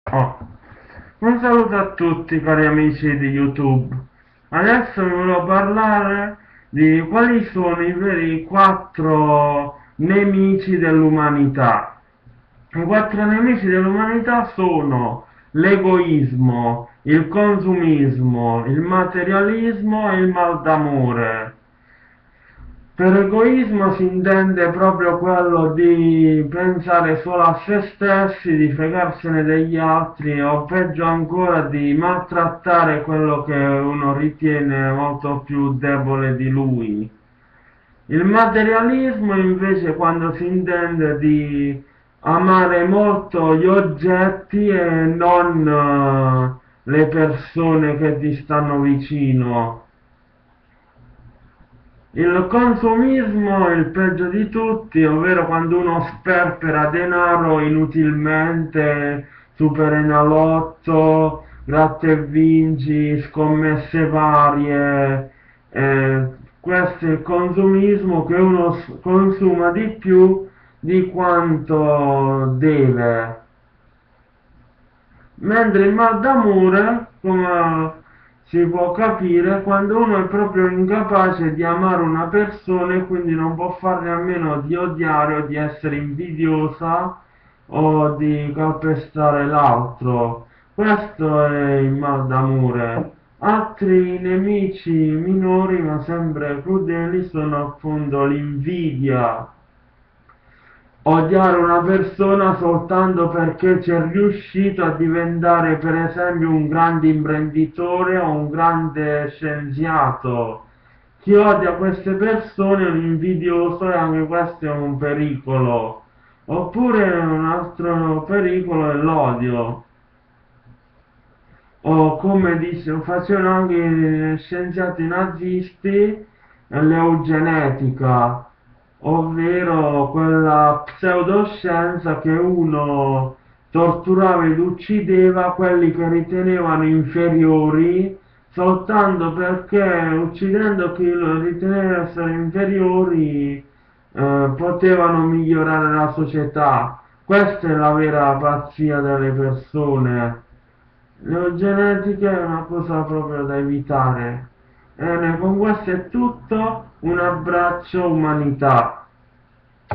I veri pericoli della mente umana
Vi elenco quali sono i veri pericoli dell'umanità: Consumismo, mal d'amore, Egoismo e Materialismo.